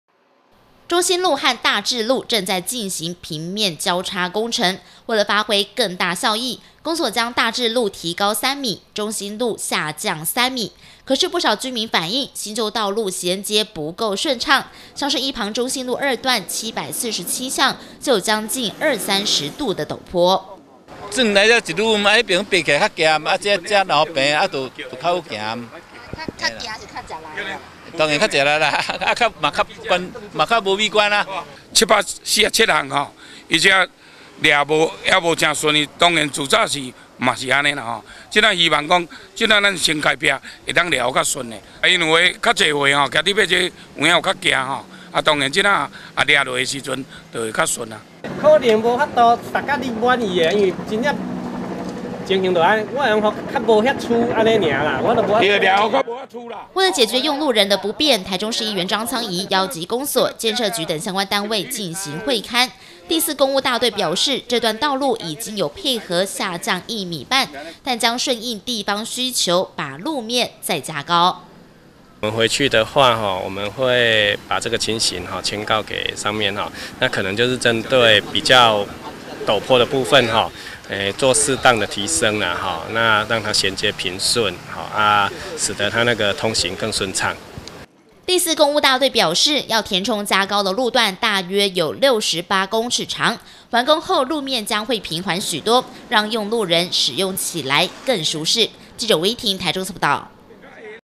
大台中新聞-大里中興路巷弄陡坡拉平會勘
大里區中興路和大智路，正在進行平面交叉工程，但一旁的中興路二段七四七巷，因為屬於舊道路，地勢比較低，不少居民都在反應，新舊道路銜接不順，造成2、30度的陡坡，老人家騎鐵馬都顯得吃力；台中市議員張滄沂，日前邀集相關單位會勘，第四工務大隊表示，會再將路面加高提升，讓用路人使用起來更舒適。「以上新聞可從哈TV APP收視」